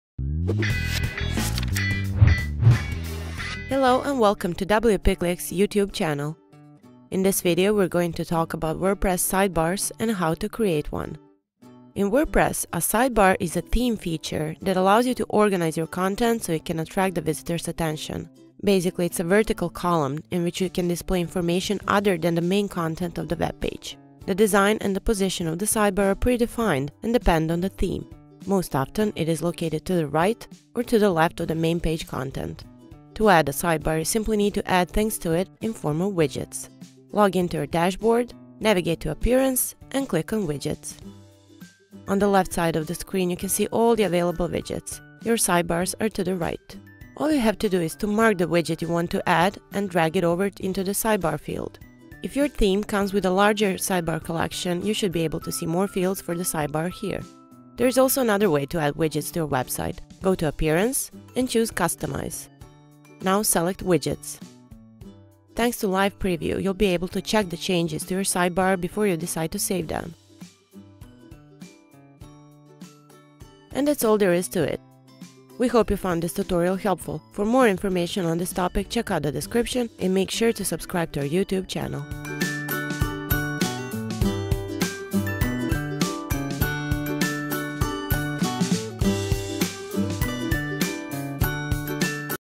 What is WordPress Sidebar and How to Create It
Curious about WordPress sidebars? Need to find out how you can edit and customize your sidebar to fit your page the best? In this video, we’re going to explain how to add and remove items from the sidebar using widgets.